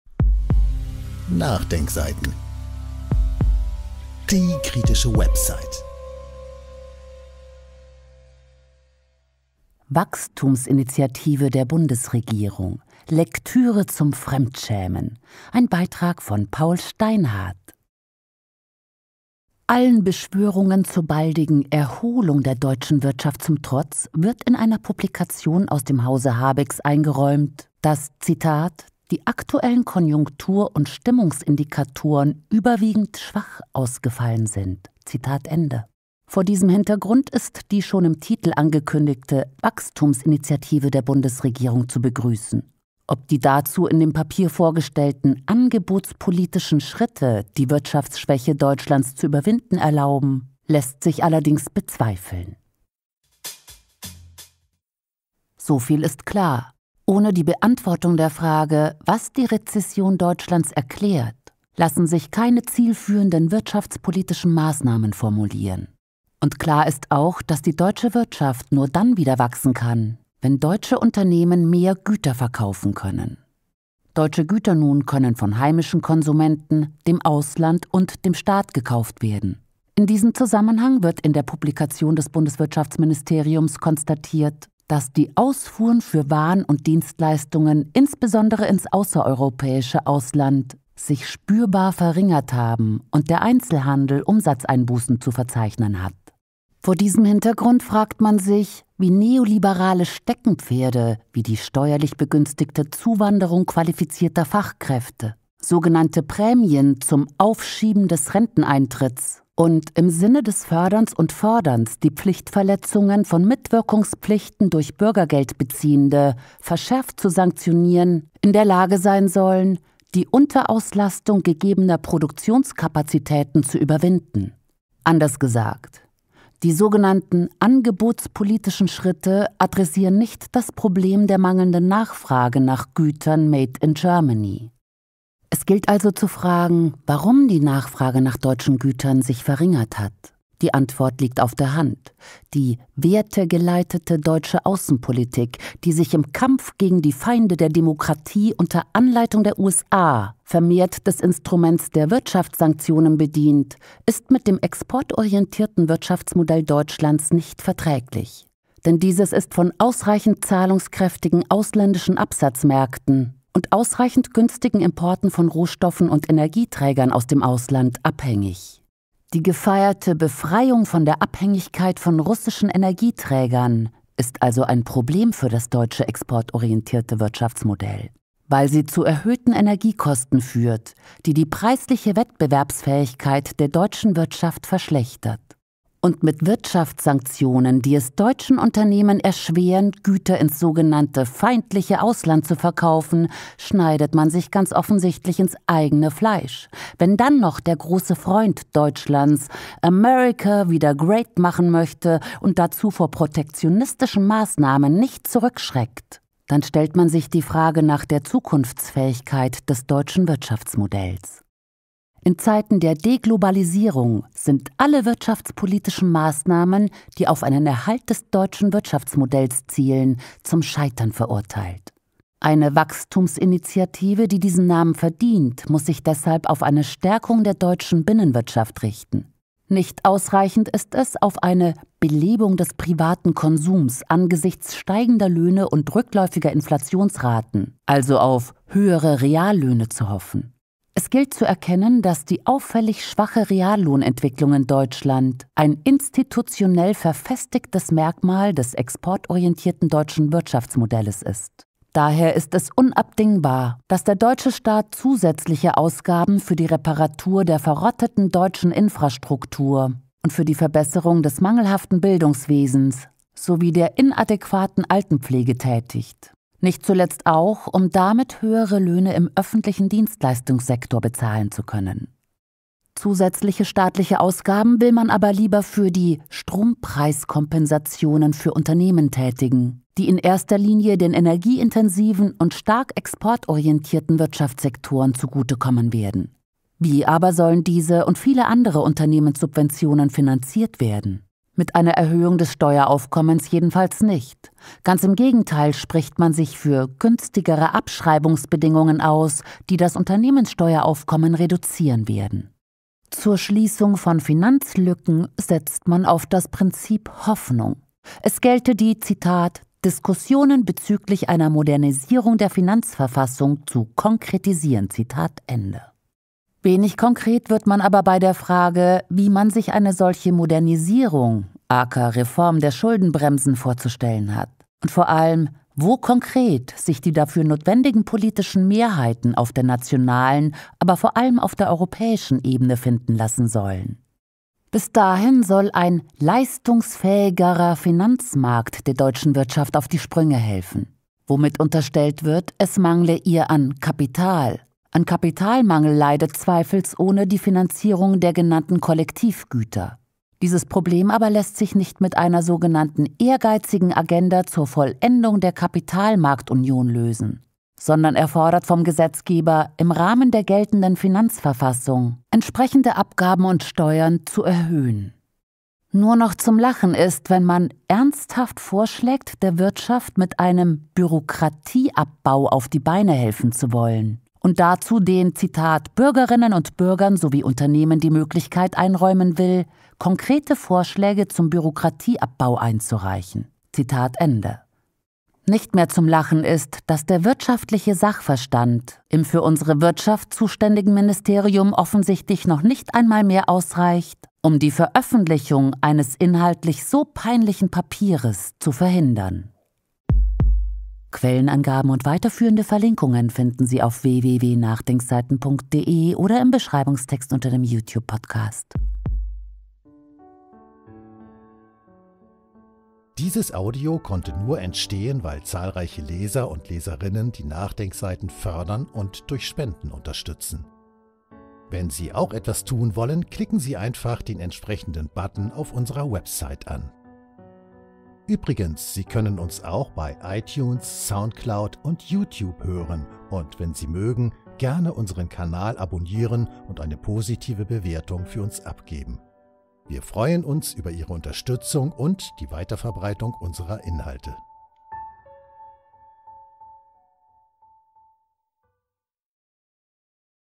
Wachstumsinitiative der Bundesregierung: Lektüre zum Fremdschämen | Paul Steinhardt | NDS-Podcast
Wachstumsinitiative der Bundesregierung: Lektüre zum Fremdschämen | Paul Steinhardt | NachDenkSeiten-Podcast | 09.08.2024

Allen Beschwörungen zur baldigen „Erholung“ der deutschen Wirtschaft zum Trotz wird in einer Publikation aus dem Hause Habecks eingeräumt, dass „die aktuellen Konjunktur- und Stimmungsindikatoren überwiegend schwach ausgefallen sind“. Vor diesem Hintergrund ist die schon im Titel angekündigte „Wachstumsinitiative der Bundesregierung“ zu begrüßen. Ob die dazu in dem Papier vorgestellten „angebotspolitischen Schritte“ die Wirtschaftsschwäche Deutschlands zu überwinden erlauben, lässt sich allerdings bezweifeln. Von Paul Steinhardt.

Artikel veröffentlicht am: 09. August 2024 um 11:11
Autor: Paul Steinhardt (Gastbeitrag)
Sprecherin: Ala Goldbrunner

Paul Steinhardt ist Buchautor und Wirtschaftsjournalist und schreibt primär über geld- und fiskalpolitische Themen. Zusammen mit Heiner Flassbeck hat er Anfang 2016 das Online-Wirtschaftsmagazin MAKROSKOP gegründet, als dessen Chefredakteur er bis Juni 2023 agierte. Zuvor hatte er über 20 Jahre im Finanzsektor in Führungspositionen im In- und Ausland gearbeitet. An der London School of Economics hat er mit einem Master of Science abgeschlossen und an der Goethe-Universität in Frankfurt/Main über Grundlagenfragen der Ökonomik promoviert.

NDS, NachDenkSeiten-Podcast, NDS-Podcast, Audio-Podcast, Wettbewerbsfähigkeit, Wirtschaftspolitik und Konjunktur, Bürokratie, Binnennachfrage, Export, Lohnentwicklung, Rezession, Schuldenbremse, Subventionen, Wachstum, Wirtschaftssanktionen